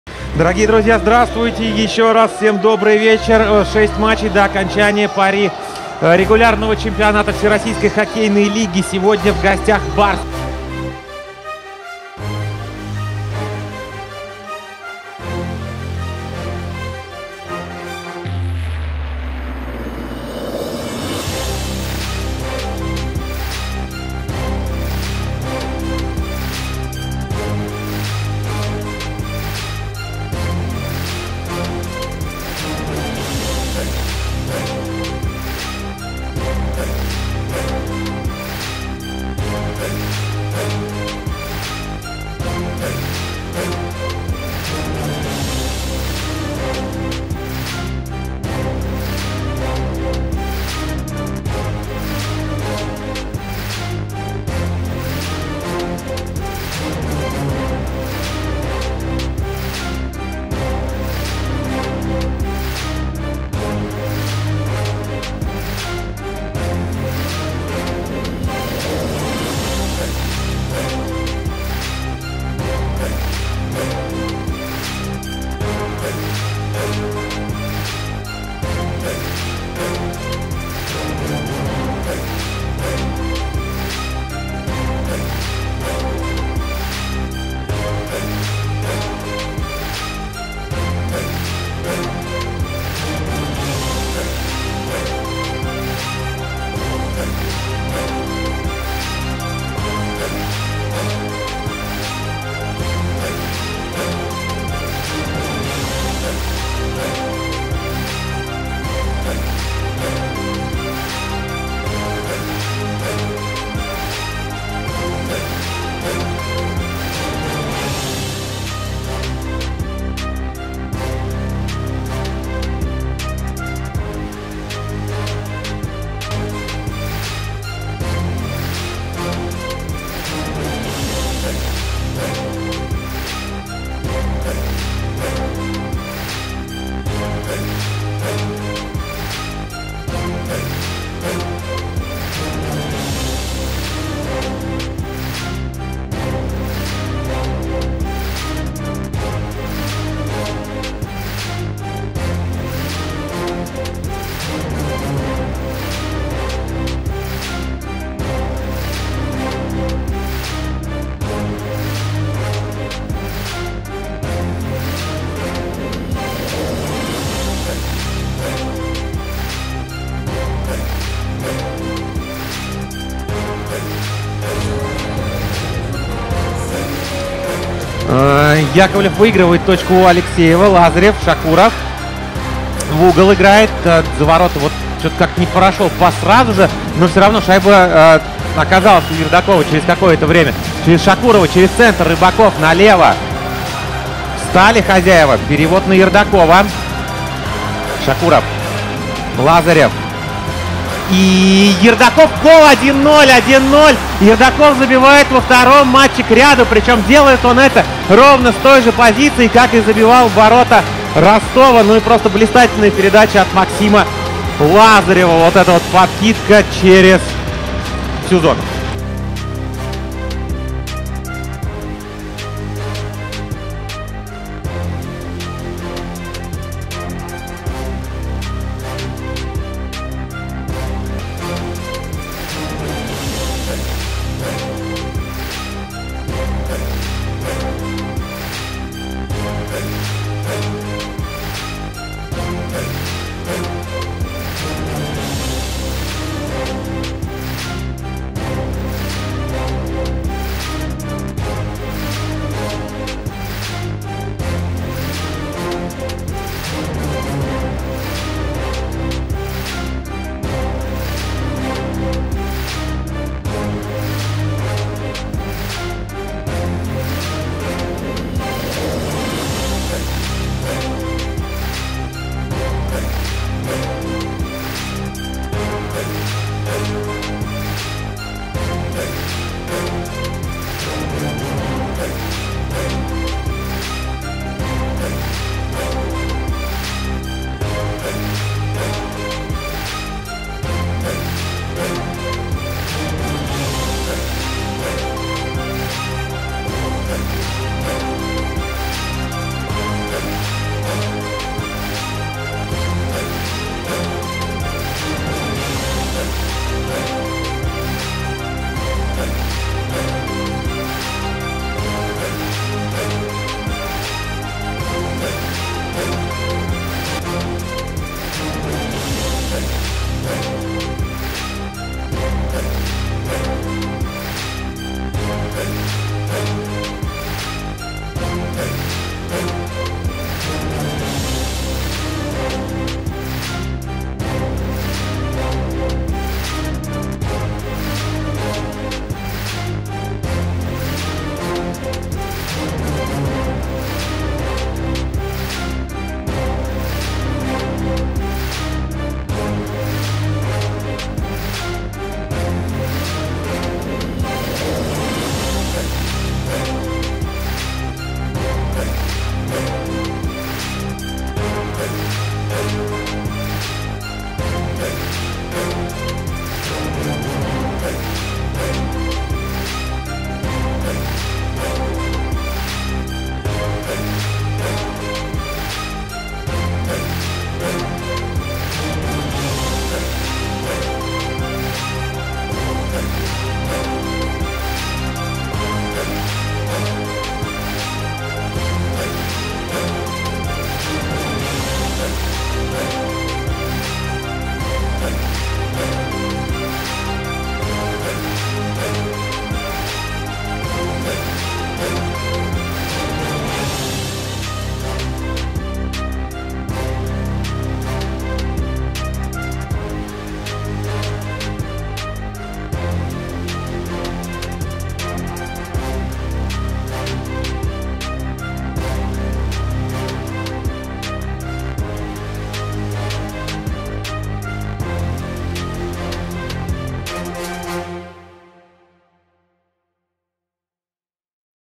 Единственная шайба Даниила Ердакова принесла АКМ победу над «Барсом»
АКМ (Тула) - «Барс» (Казань) 1:0

Единственная шайба Даниила Ердакова принесла АКМ победу над «Барсом». Сергей Большаков оформил «сухарь»